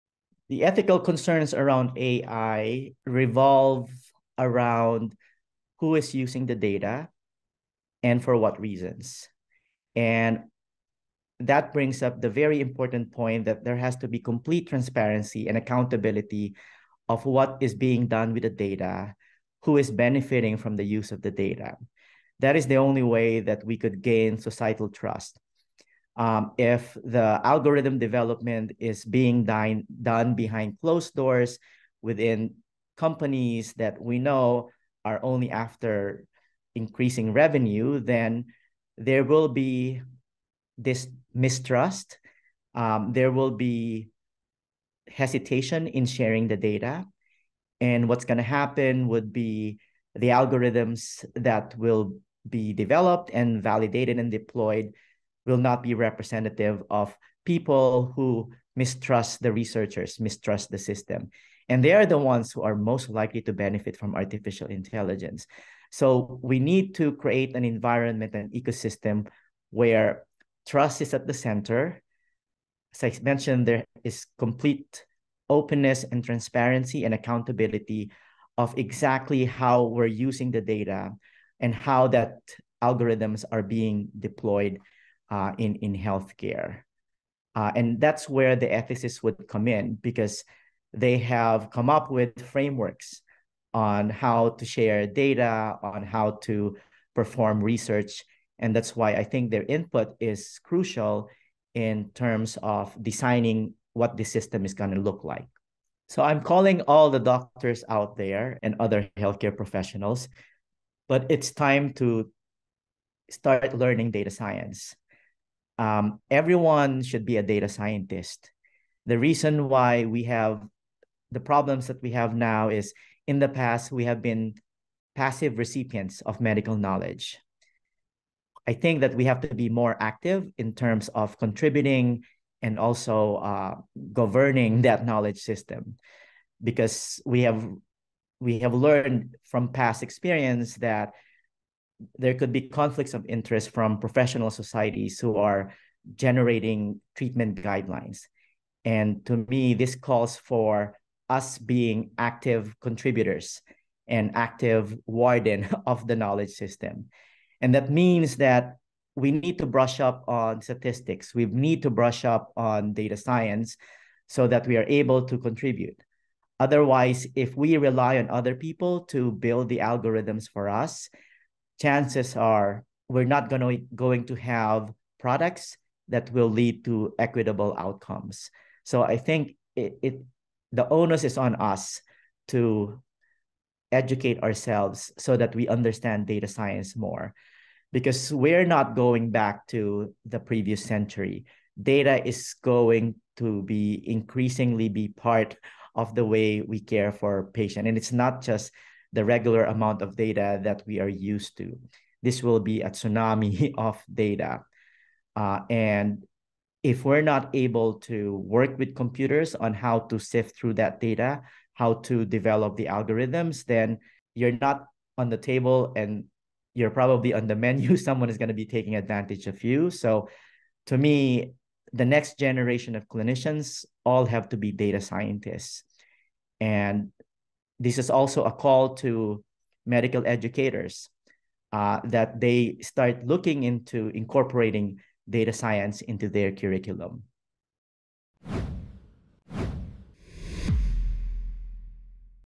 Getting Patients to Trust AI
In this video, Leo Anthony Celi, MD, MPH, principal research scientist at the MIT Laboratory for Computational Physiology (LCP) and an intensive care unit physician at Beth Israel Deaconess Medical Center in Boston, explains the importance of gaining patient trust in order to generate accurate artificial intelligence (AI) models. Celi also calls for all medical professionals to learn data science, so that they can continue to be "at the table."